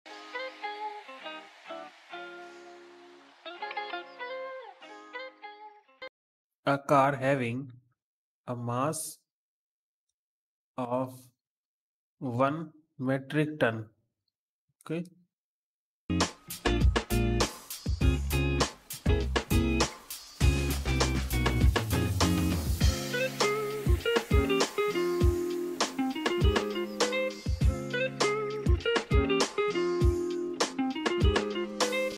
A car having a mass of 1 metric ton is moving with a speed of 30 m/s. It suddenly applies the brake…
A car having a mass of 1 metric ton is moving with a speed of 30 m/s. It suddenly applies the brakes and skids to rest in a certain distance d. The frictional force between the tyres and road is 6000 N. What is the value of d? a) 25 m b) 50 m c) 75 m d) 100 m